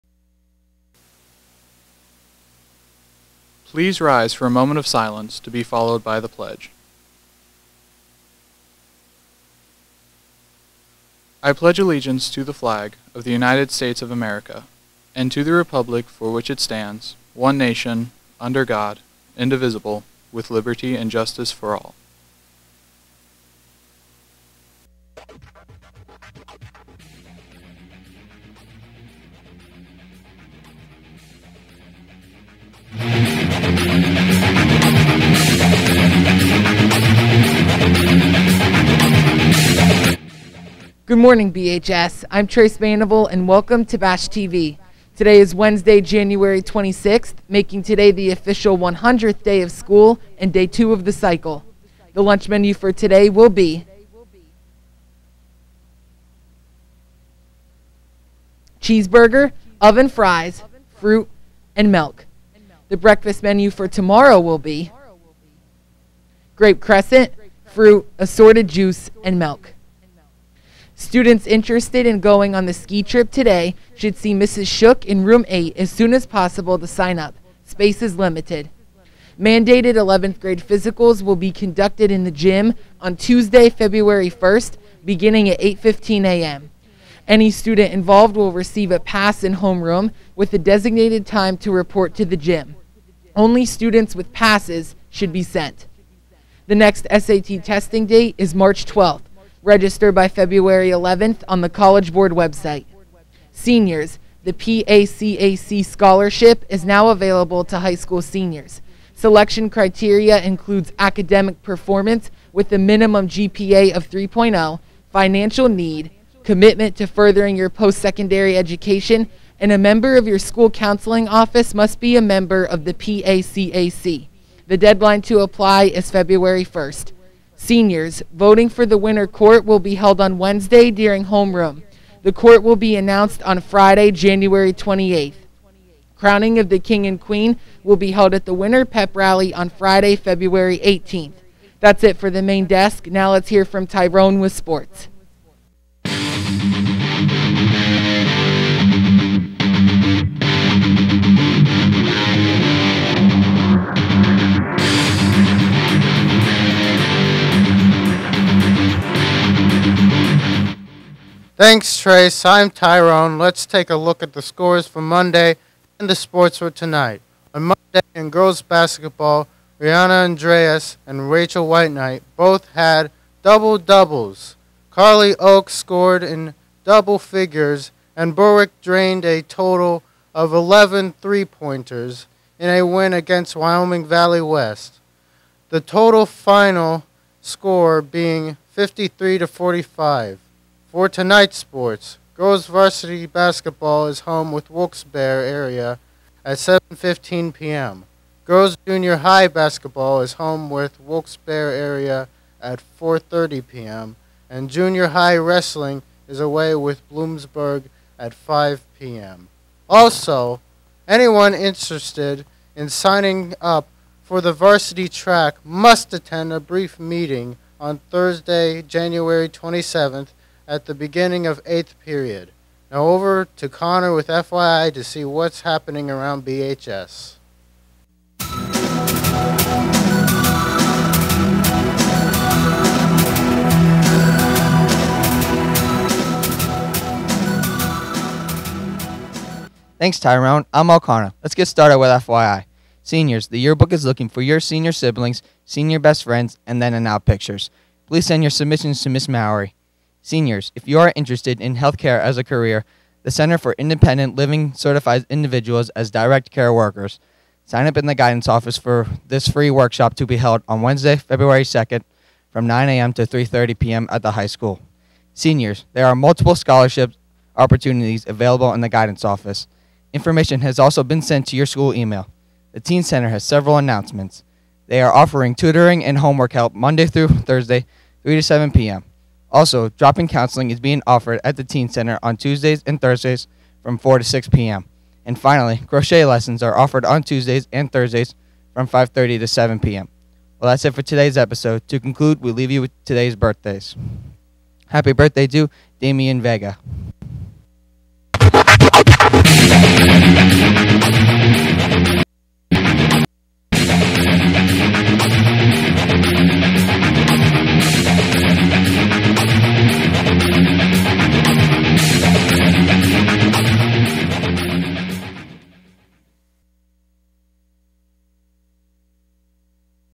BASH TV for Wednesday, 1/26/2022
Videos from Berwick High School in Berwick, PA.  Announcements from Berwick Area High School and the Berwick Area School District.